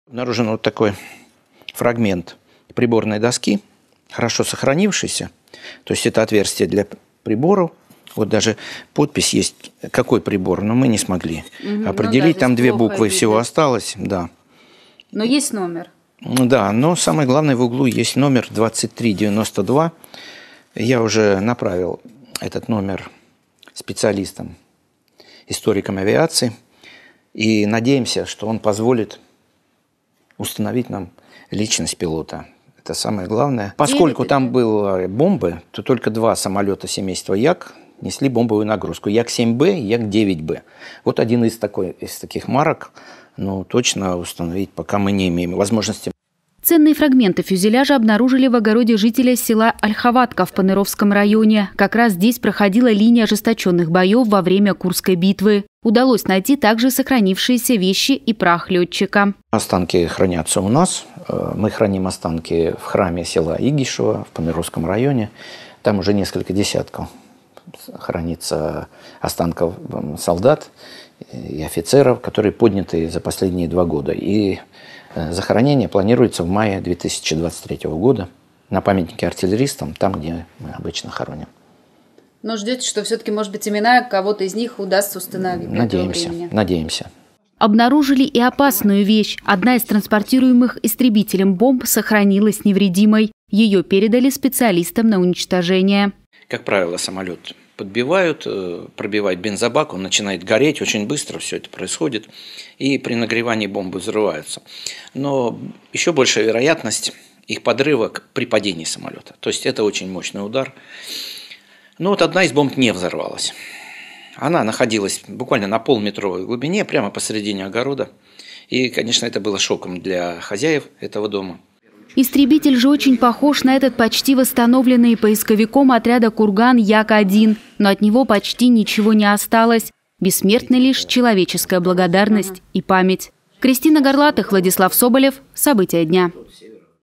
В Курской области в огороде нашли истребитель с останками пилота
Поисковики отряда «Курган» обнаружили обломки истребителя, принимавшего участие в Курской битве, прямо в огороде жителя Поныровского района. А ещё — неразорвавшуюся бомбу. Её передали для ликвидации специалистам, а модель самолёта и личность пилота ещё предстоит установить.